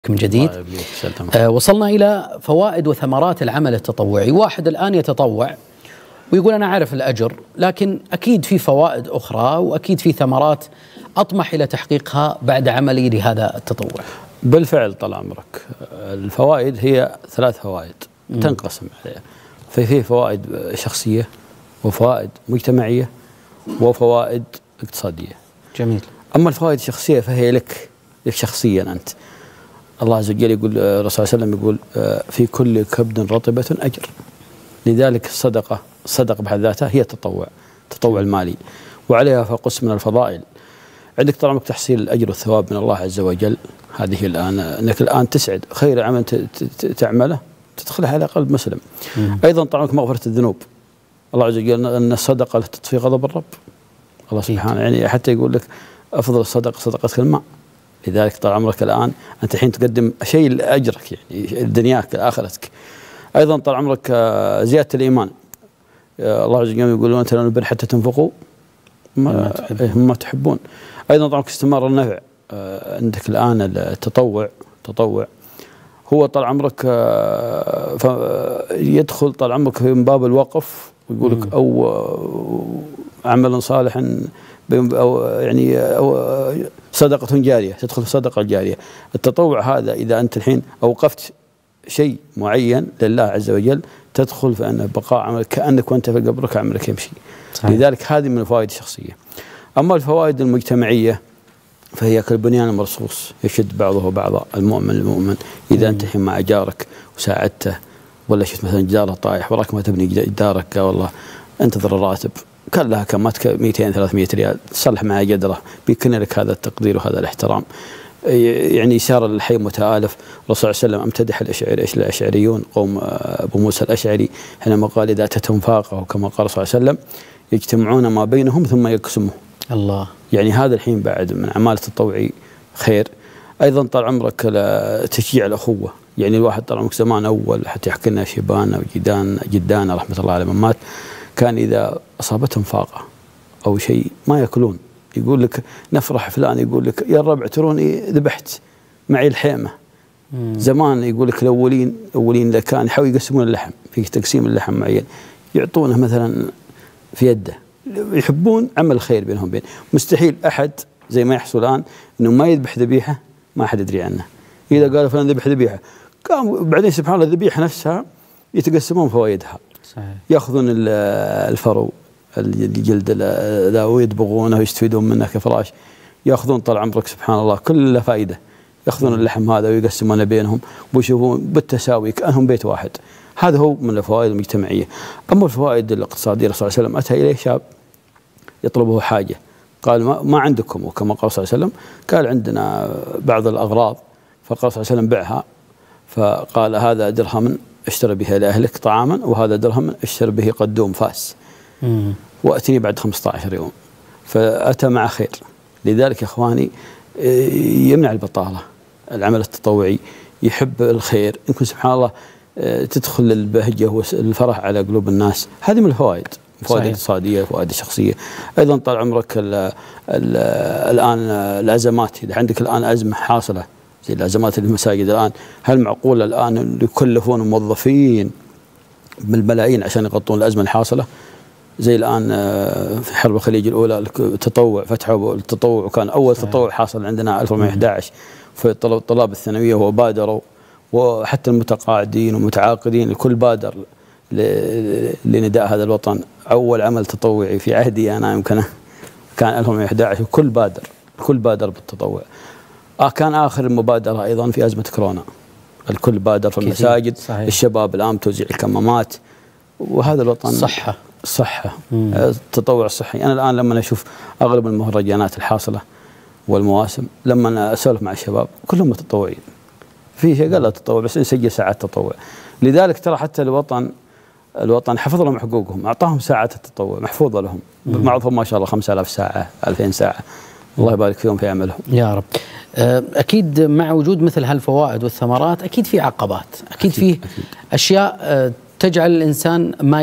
فوائد وثمرات التطوع على الفرد والمجتمع وعلى الاقتصاد الوطني. أ/ عوض الصليم
تقديم: عبدالحكيم السيف

ومع ضيفنا:

أ/ عوض الصليم
يعرض:
الجمعة 2:20 م
13 قناة آمنة لأسرتك!